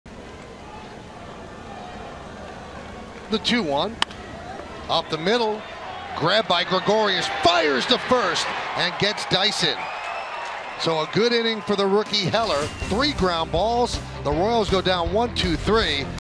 NYY@KC: Gregorius robs Dyson with a slick play
Didi Gregorius ranges up the middle to field Jarrod Dyson's grounder, then spins and fires to first base in time to record the out